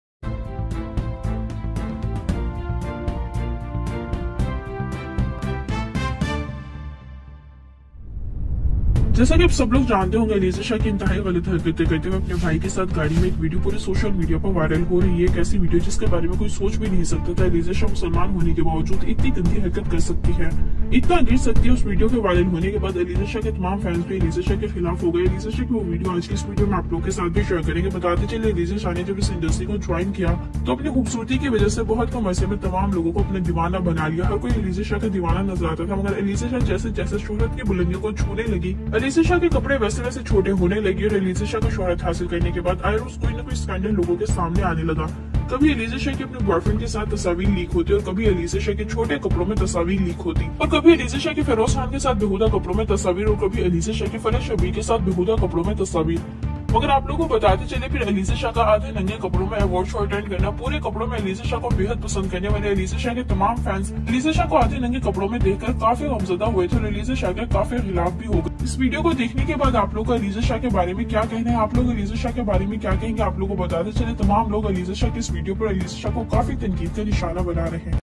Breaking News! Alizeh Shah's Private Video Leaked from Her Car | Alizeh Shah New viral video
Breaking News! Alizey Shah's private video has just been leaked from her car! In this video, we get a glimpse of Alizey's personal life and the controversy that has sparked since the video went viral on social media. Watch the full story and find out what Alizey has to say about the whole situation.

Your Searching Quires:

aiman khan and alizeh shah and kinza hashmi drama,

Your Searching Quires:

dulhan aur aik raat alizeh shah episode 4 full,